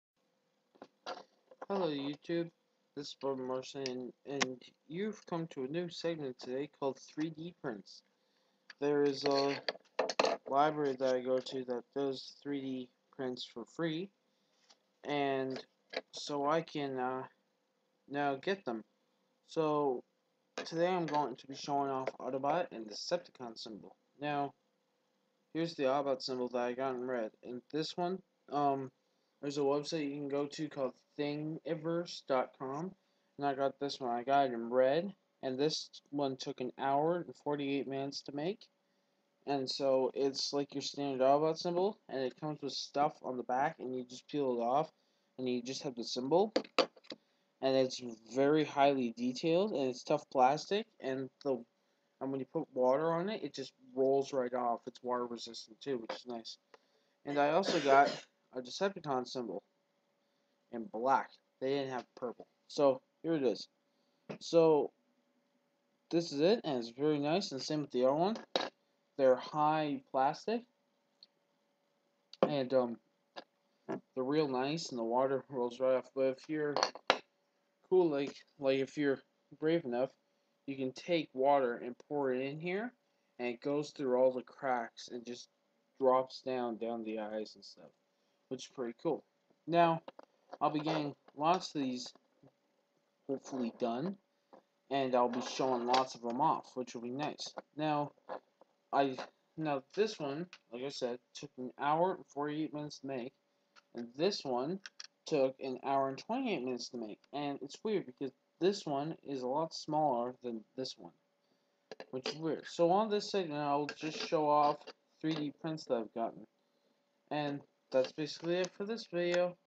3-D Prints
This video shows a new series of videos to come of 3-D printed objects.